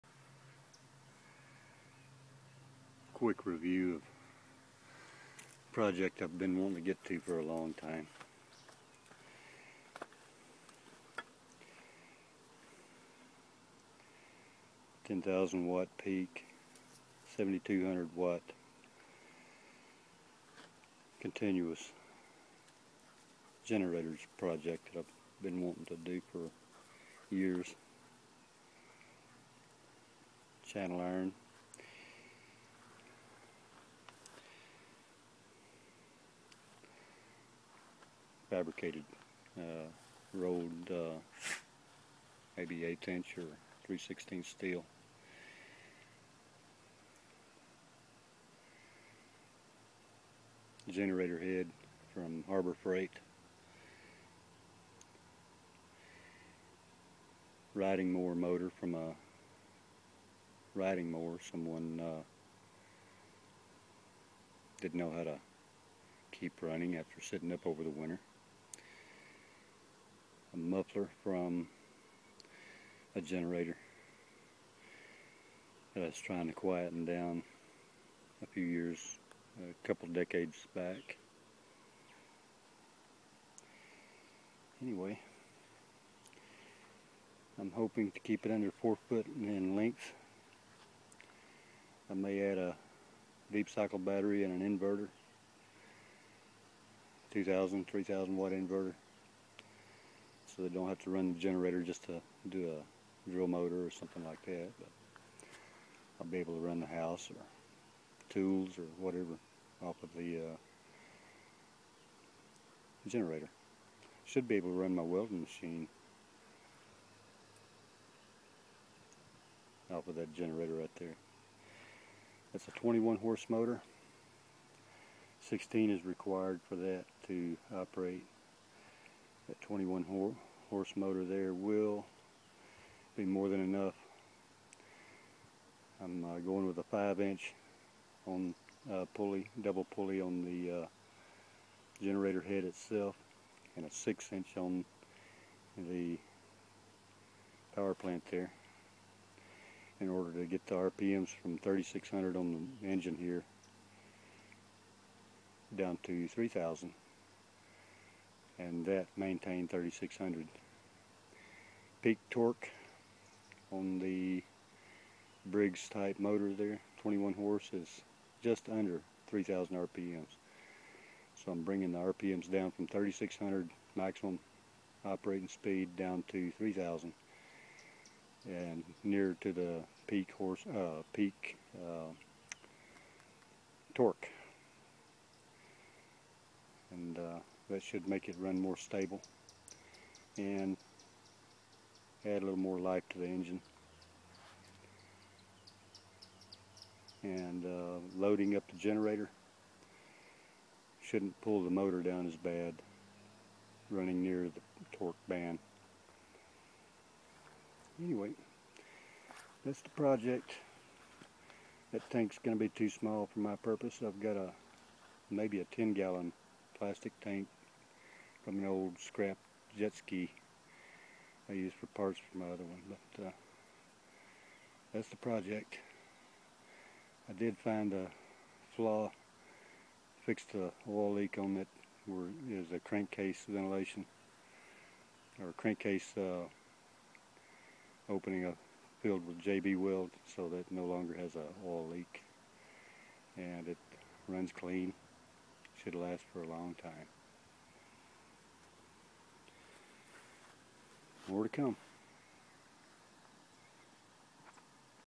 DIY custom 10,000 watt generator project.
These old bones have a problem with jerking on a rope starting system to start a generator so I am building a generator with electric start. With a continuous 7,200 watt rating that requires a 16hp motor, I will be able to use the 21hp rated motor with a 5" pulley on the generator head and a 6" pulley on the power plant which reduces the power plant RPM from 3,600 to 3,000. The peak torque on the Briggs motor is just under 3,000 rpm's and that should also stabilize the motor loading rpm drop when with heavy draw. Note the cheap, vertical shaft Briggs V twin 21hp motor was so cheap, it was free. It had a small hole in the crank case that a little JB weld fixed and it now runs clean and likely has a good bit of life left in it. The generator head on special and with a coupon, cost under $270 with shipping charges from Harbor Freight. The steel channel everything will be mounted on is something salvaged at least a decade back. The RV generator muffler is left from an earlier generator I had a couple decades back. Total investment in this generator will be under $500 and anything that compares price wise would be well over a thousand. It has taken me a long time to gather what I needed for this project but I now have it all in one place and will try to document the process as I proceed. It will be a long running process by the time I add battery pack and inverter to handle short term light loads such as drill motors or grinders. For now, just having an easy to start portable power supply that will handle the load of anything I may need is well worth the effort.